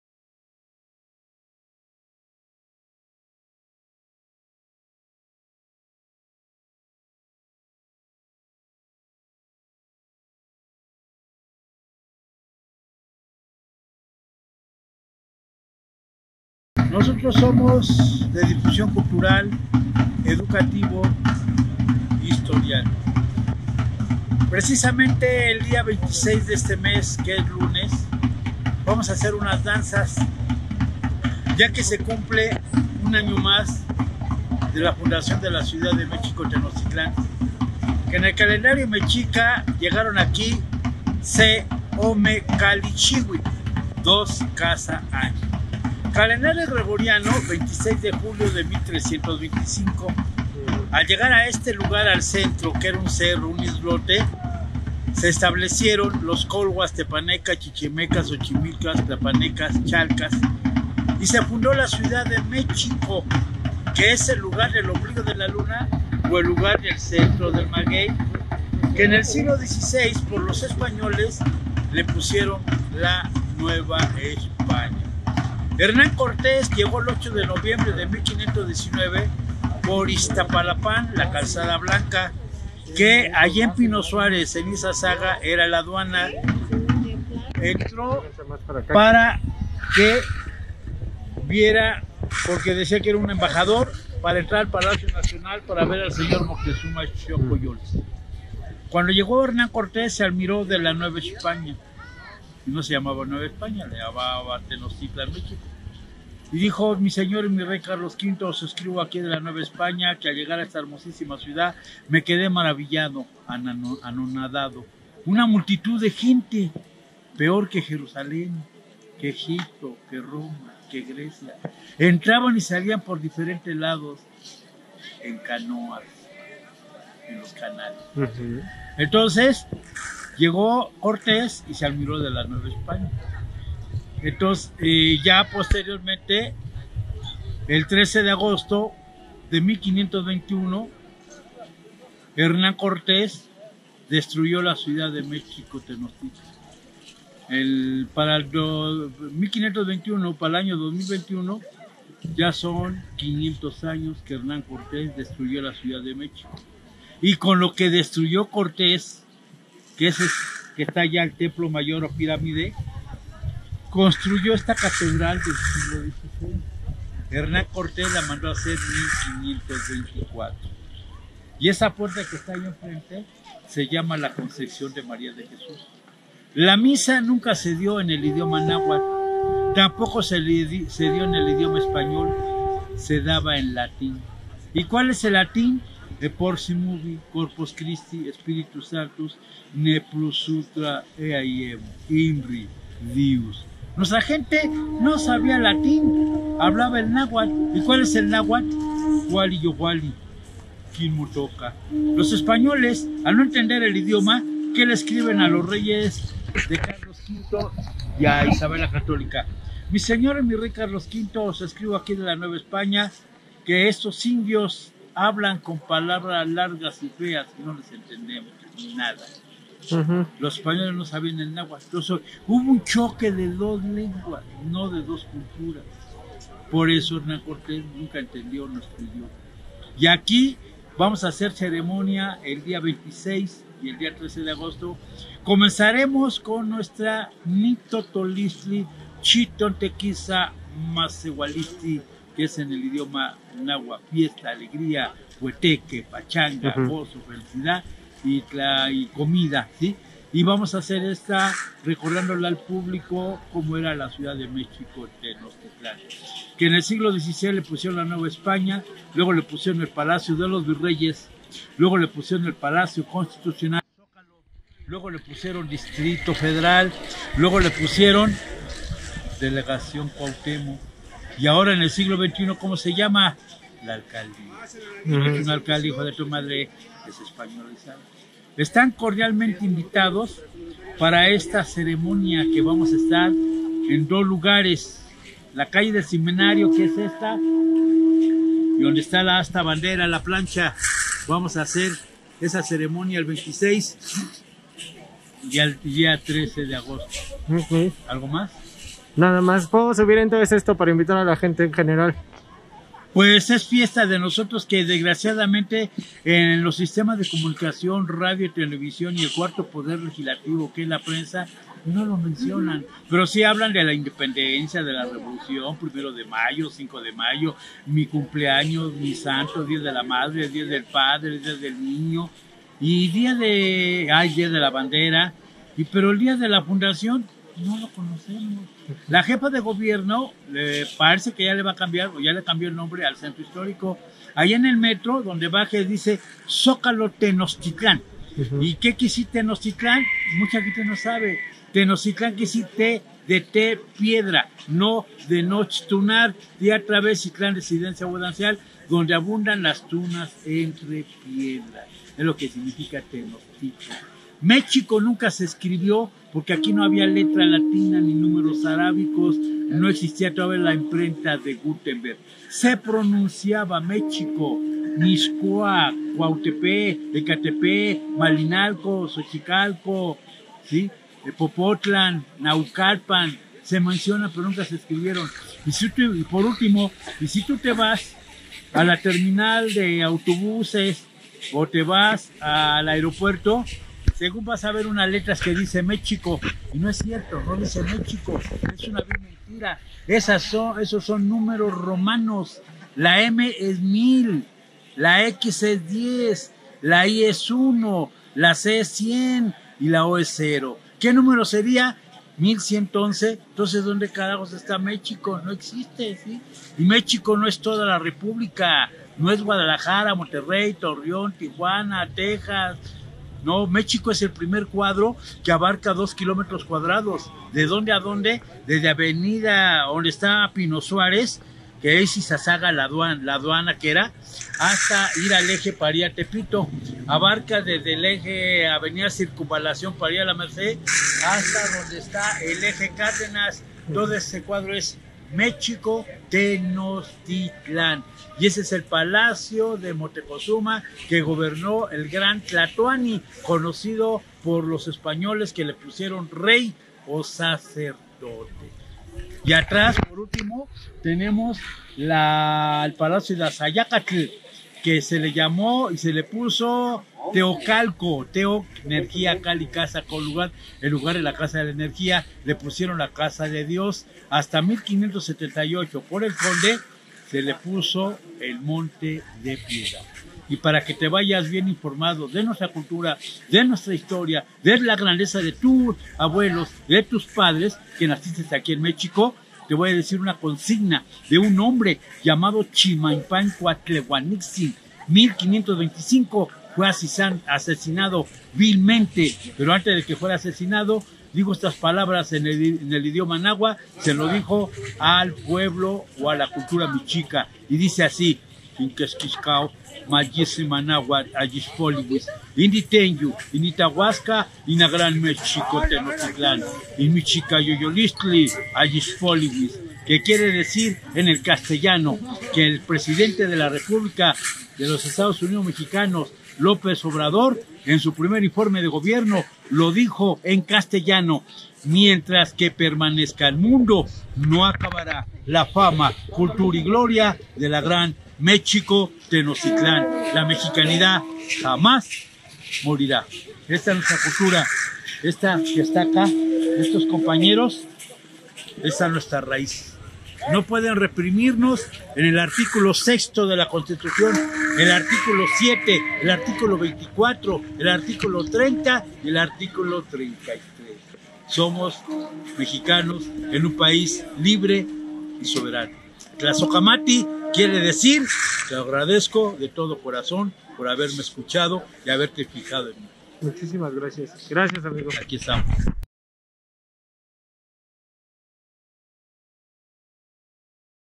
MIENTRAS QUE PERMANEZCA EL MUNDO NO ACABARÁ LA FAMA, CULTURA Y GLORIA DE MÉXICO TENOCHTITLÁN
Esta es una pequeña explicación de porque es importante la celebración del 26 de Julio para México, encontraremos algunos significados de palabras y datos interesantes. En lecturas nocturnas estamos recopilando voces e ideas para comprender quienes somos, de donde venimos y el conocimiento para poder vivir con libertad en esta realidad.